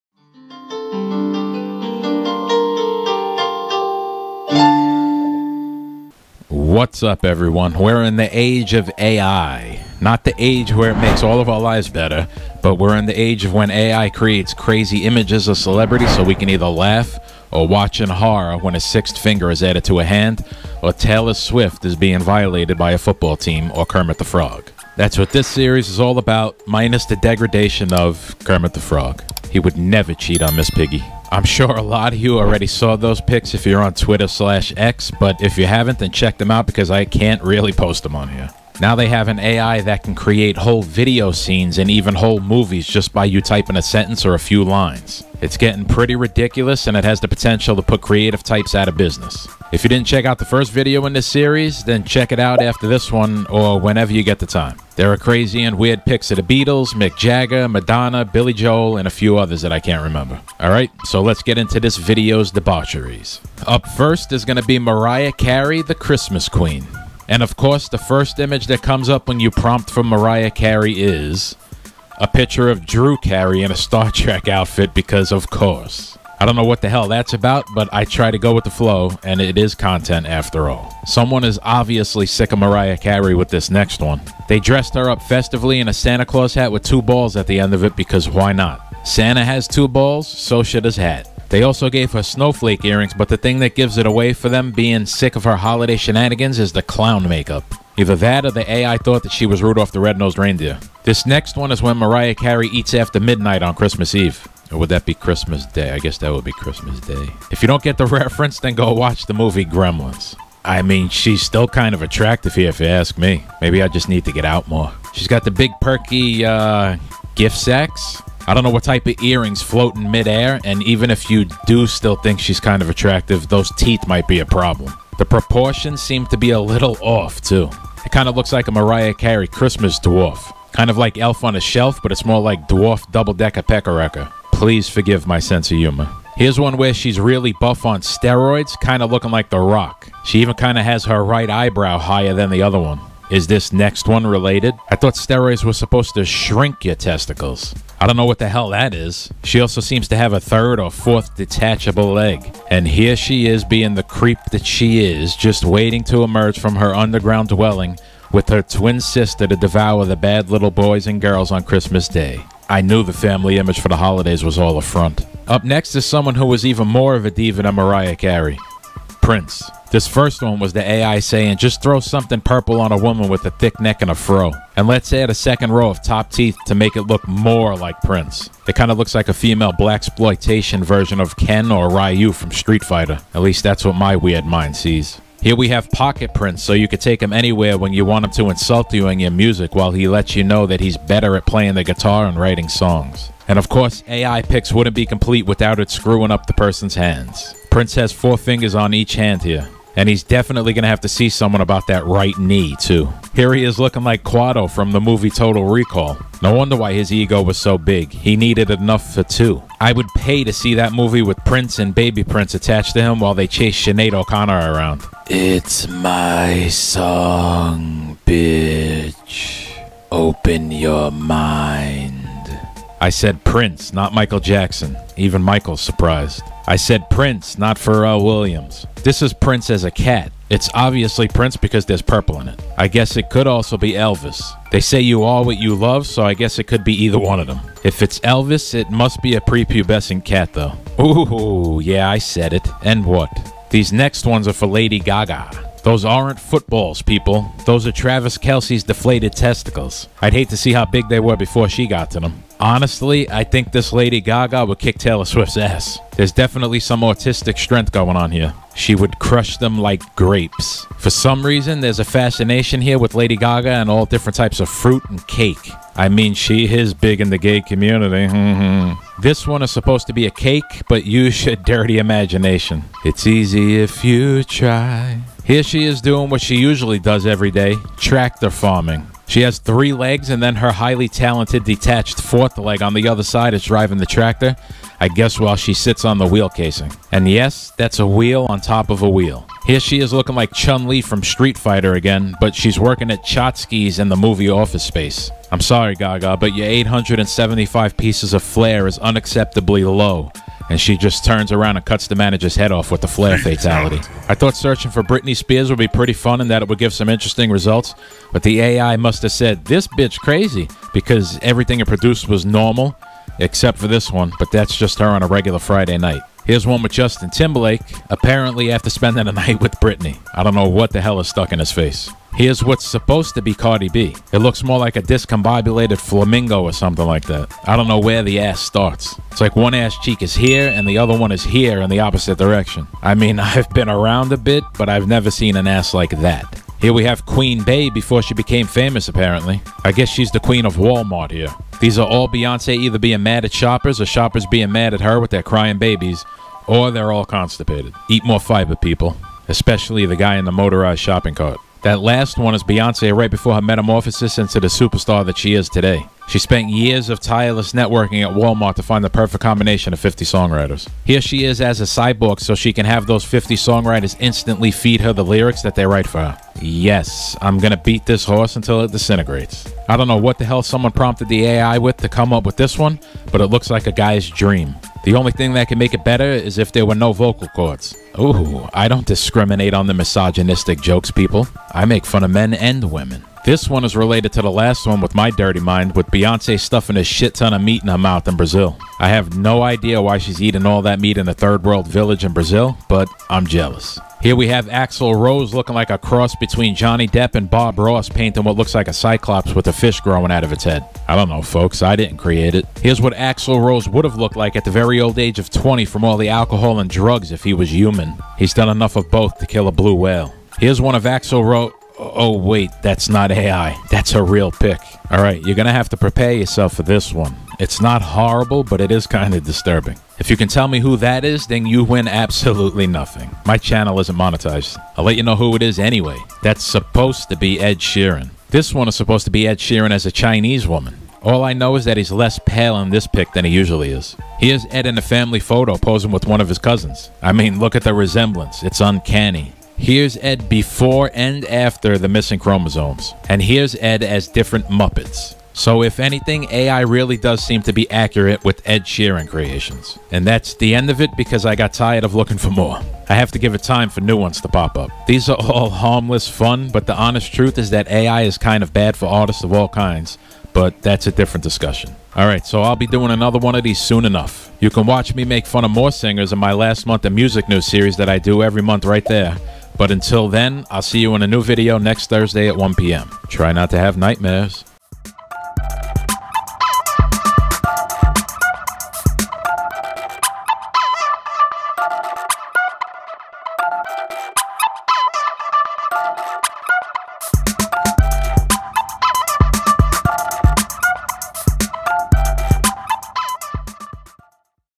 AI Generated Famous Singers #2 | Keys To The Castle Music
I searched high and far, scoured the internet and the universe, travelled millions of miles...all from the comfort of my chair...to find AI generated pictures of famous singers! ...and they're all as weird as you might have thought they'd be!